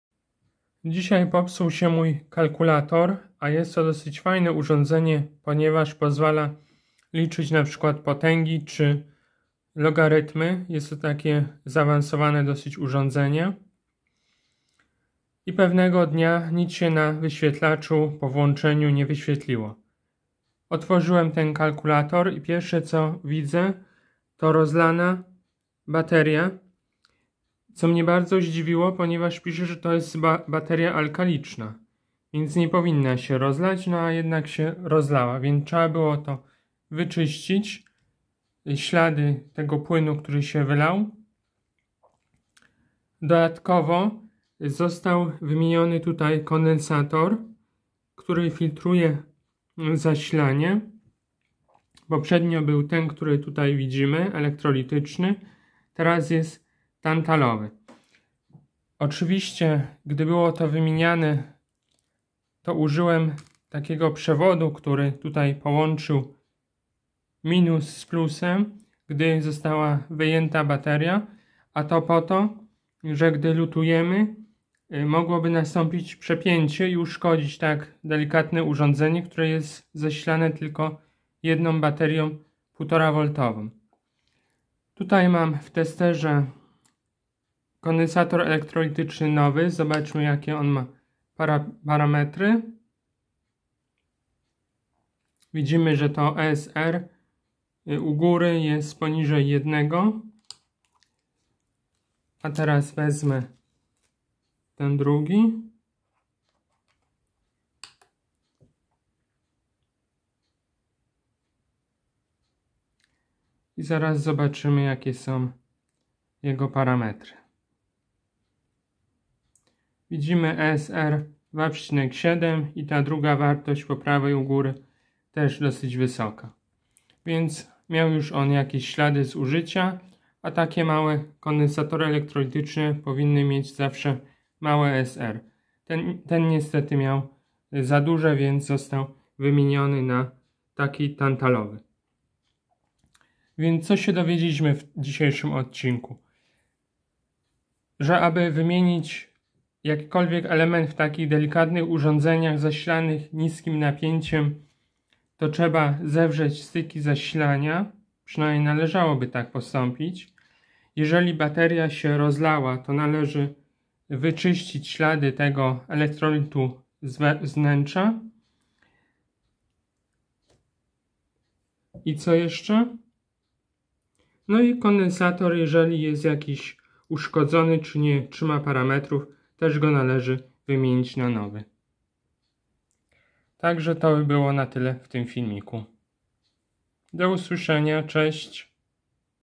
Naprawa kalkulatora. Przywrócenie sprawności sprzętu.
Filmik przedstawia wnętrze kalkulatora.
Omawiam proste sposoby poradzenia sobie z usterkami.